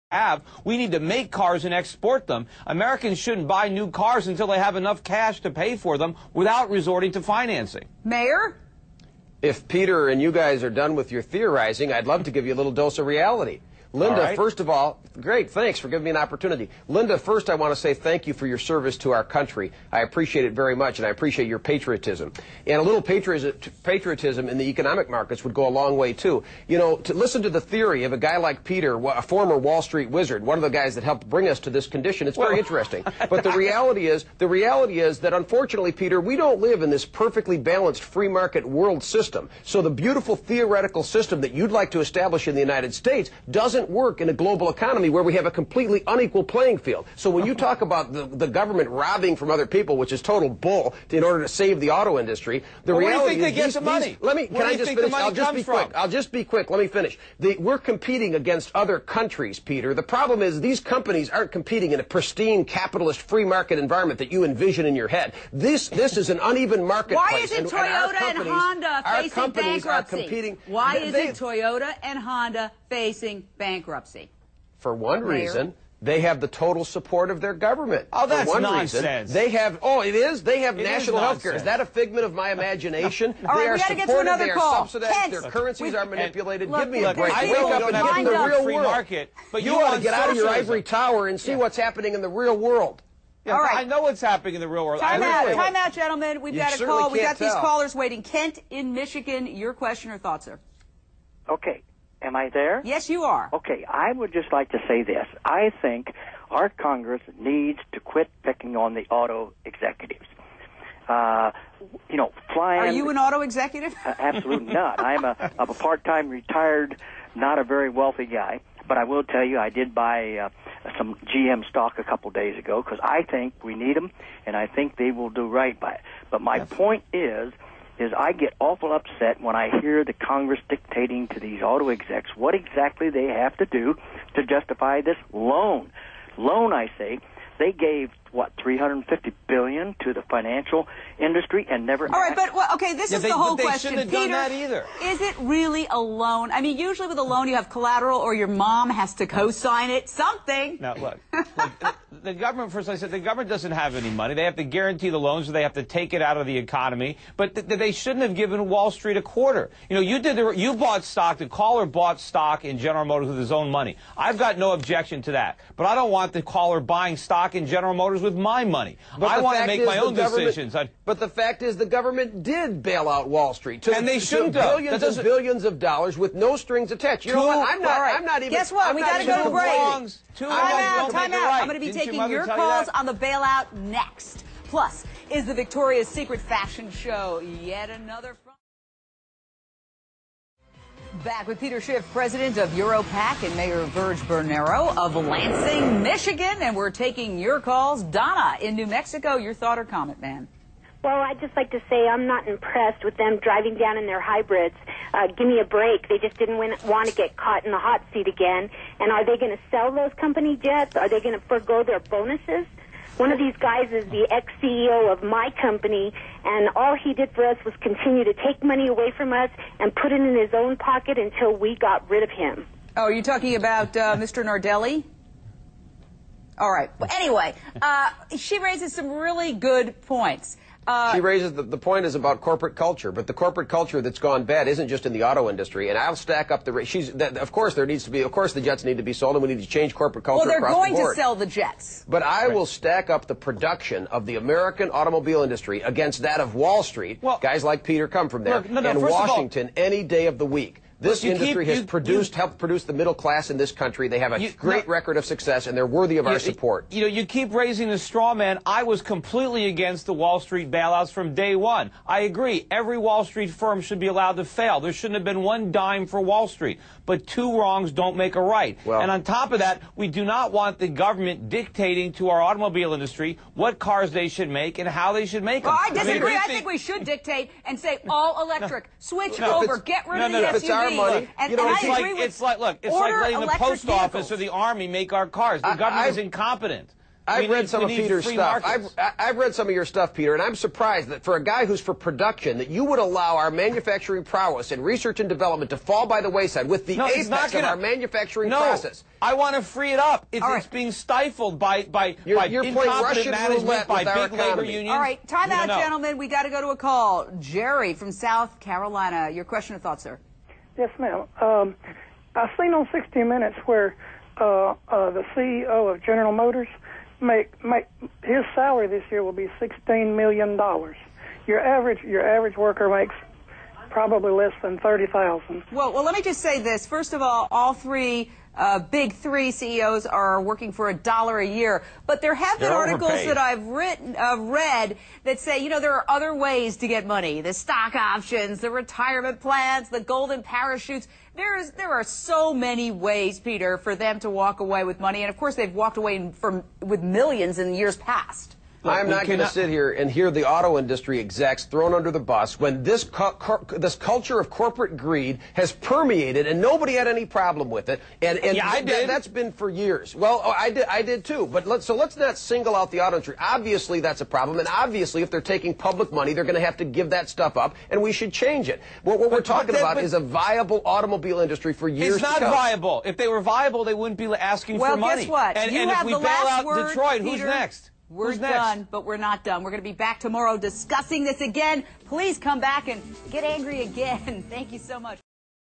Peter Schiff on CNN Auto Bailout Intense Debate! 12-4-08 part 2/2
Peter Schiff, on the auto makers fascist bailout, no one says it better. Wake up...time is short. The economic storm is coming your way!!!

"Liberty, when it begins to take root, is a plant of rapid growth." ~George Washington

In pursuit of liberty of liberty for my children and yours,

robpatozz